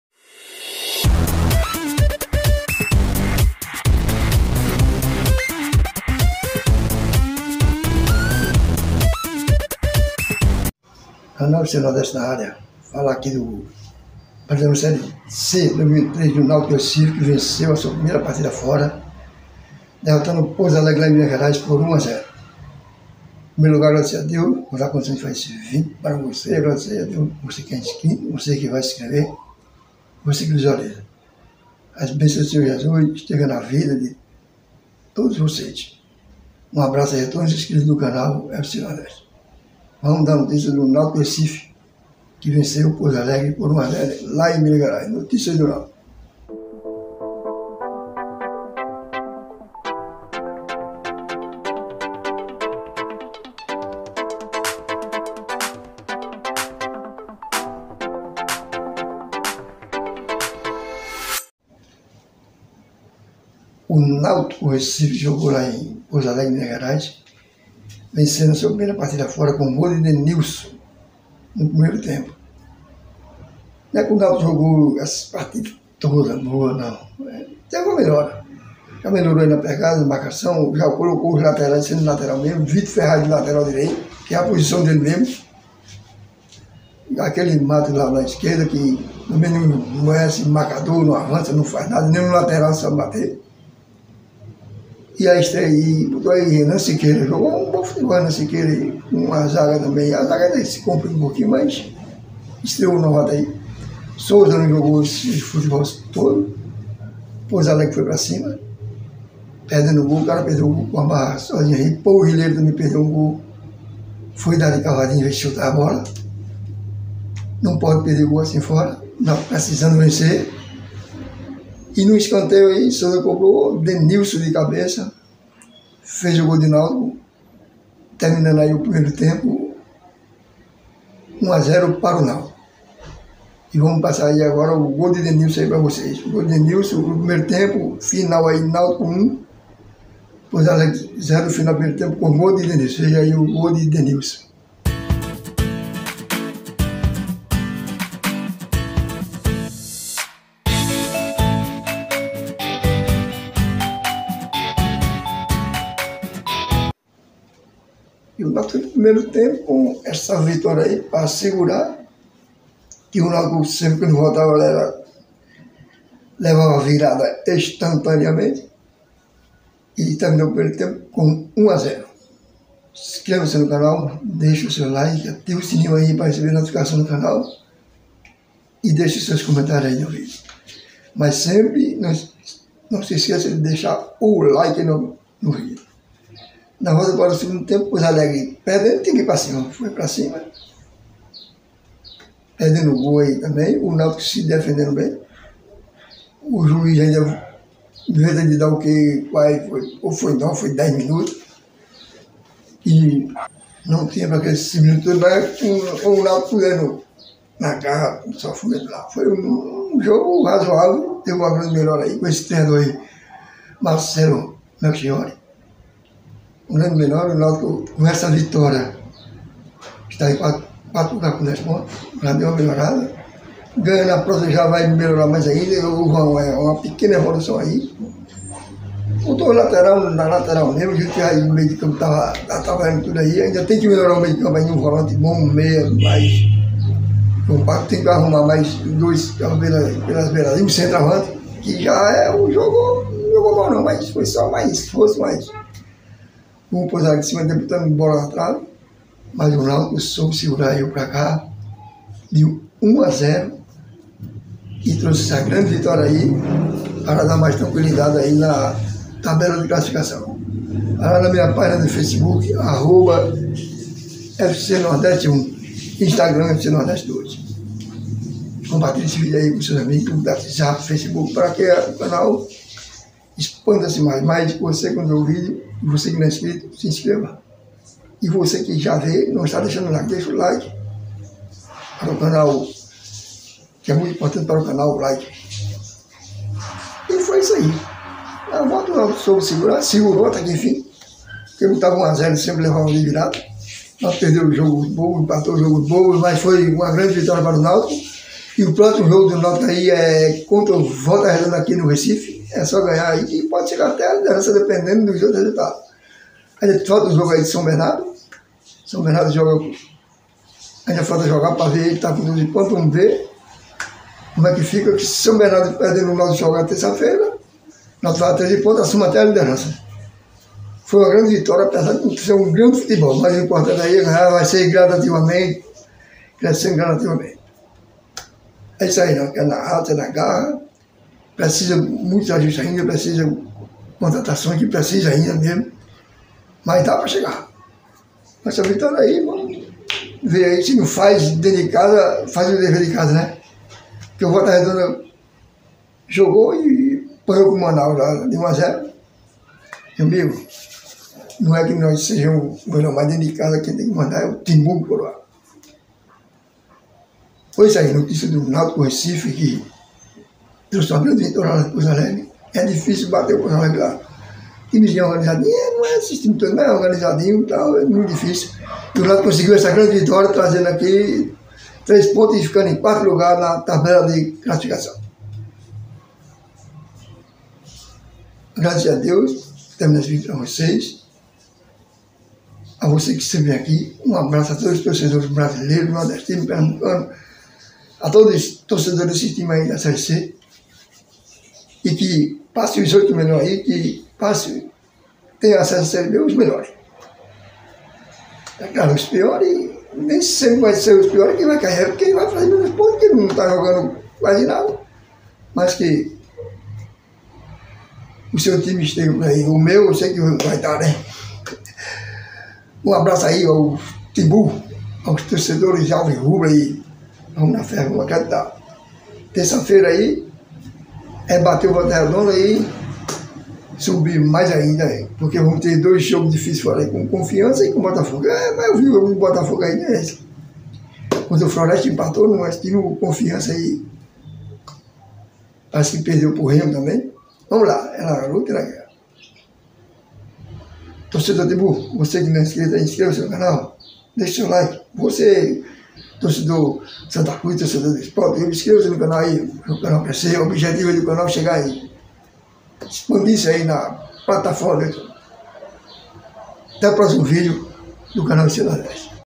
Timbu, venceu fora
CANAL FC NORDESTE: Série C2023. O náutico jogando pela sexta rodada da competição lá em Minas Gerais derrota o Pouso Alegre por um a zero. Primeira vitória fora de casa do timbu. que teve a estreia do treinador Fernando Marchiori. Com a vitória o náutico assume a quinta colocação com dez pontos ganhos. O gol do timbu foi marcado por Denílson de cabeça. Próximo jogo do náutico terça-feira nos aflitos pela sétima rodada da competição contra o volta redonda do Rio de Janeiro. Que Deus abençoe todos vocês. Muito Obrigado.